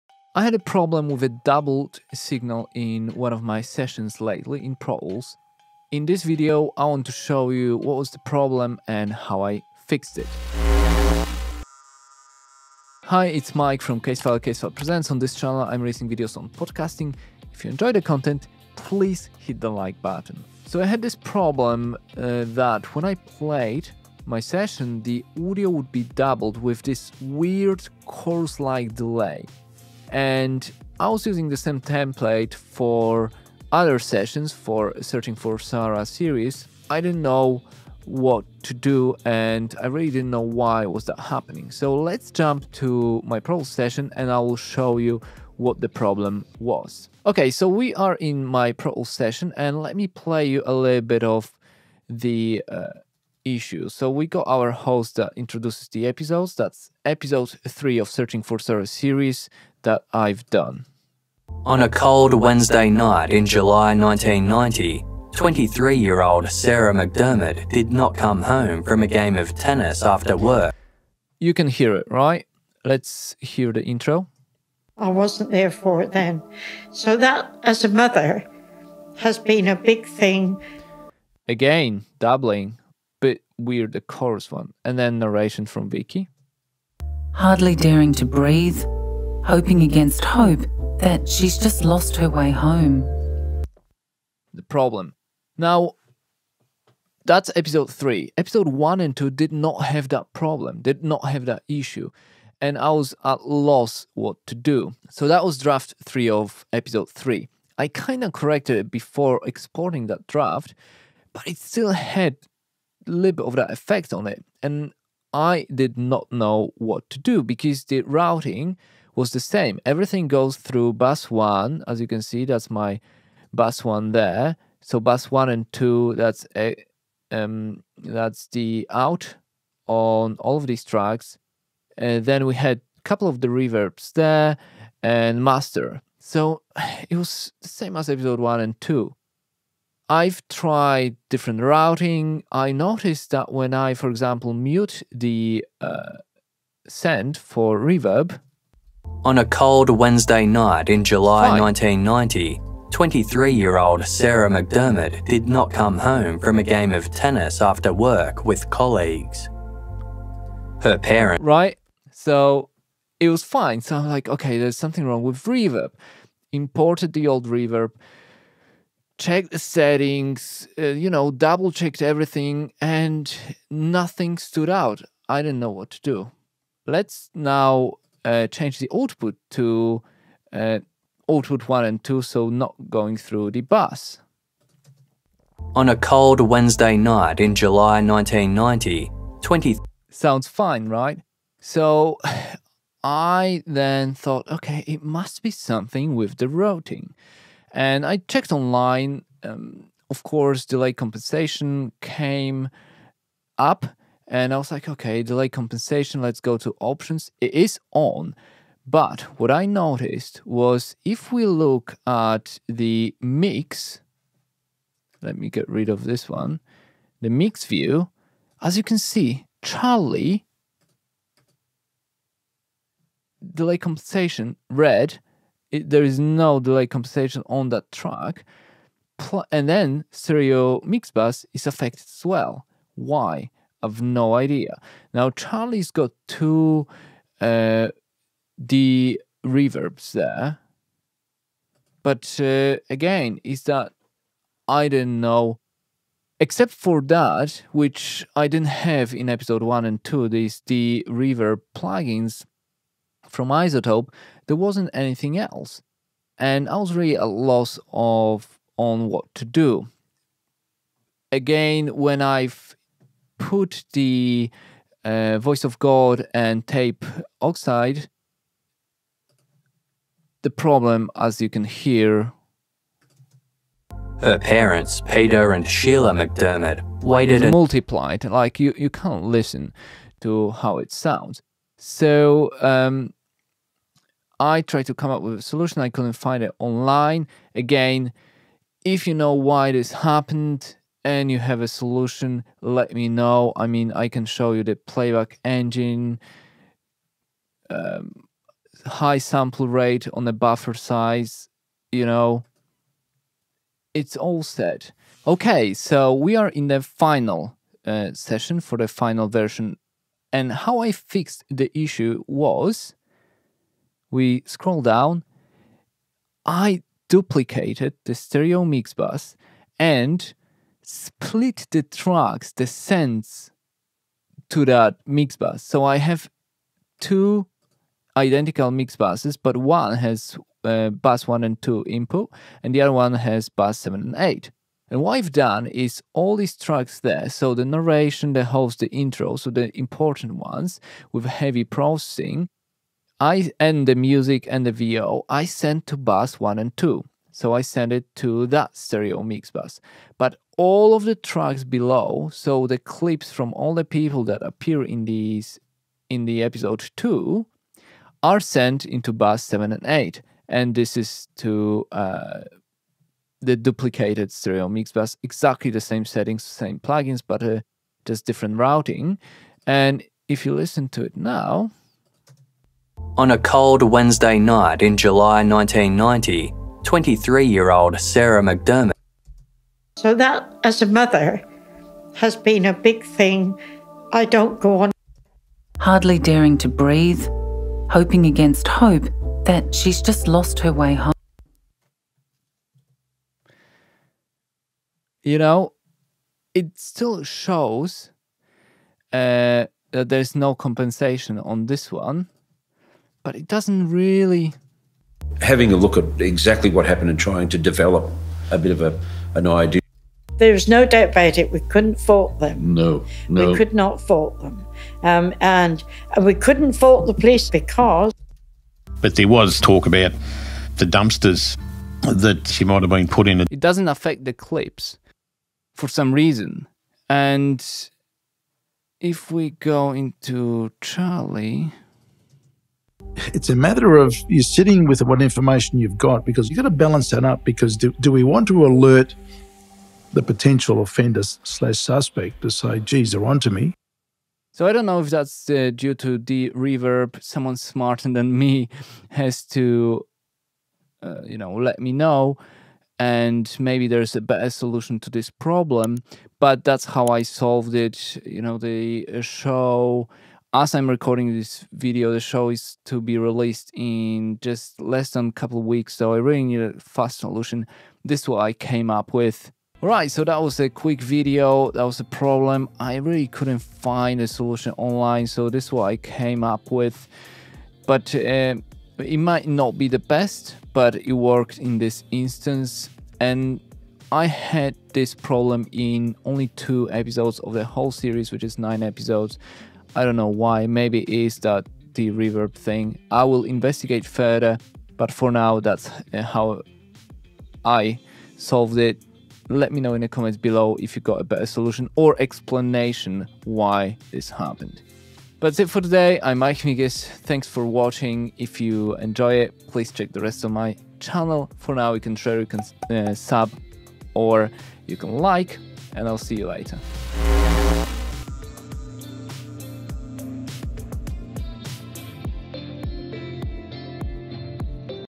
Issues With Podcast Editing? Pro Tools Delay Compensation FIX!
I had a problem Delay Compensation in Pro Tools a few weeks back. It doubled signal in one of my sessions, and in this video, I wanted to show you how I fixed it.

00:00 Intro
01:00 The problem
02:30 Trying the fixes
03:30 Delay Compensation
05:00 Solution

My podcast production studio kit

Video recorded and edited by Matt Migas & Piotr Migas